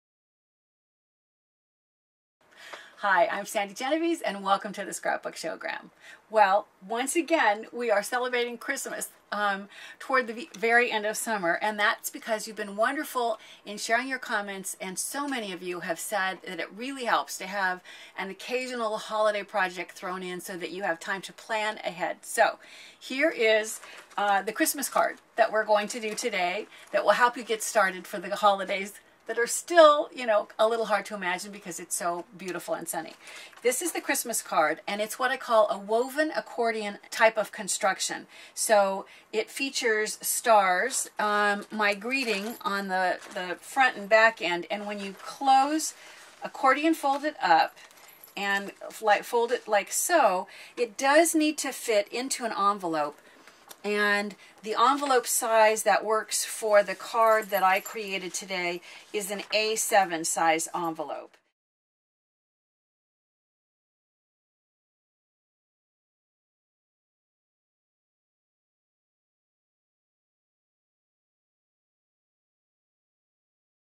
Super Dimensional Christmas Card with Sandi Genovese
We're going super dimensional this week. In response to so many of your requests, we're doing a Christmas card well before the holiday, and it's called a WOVEN ACCORDION CHRISTMAS CARD. Of course it's still summer, but as is almost always the case, the techniques can be used for any occasion. The "wow factor" will be incredible, no matter what the occasion or who receives it.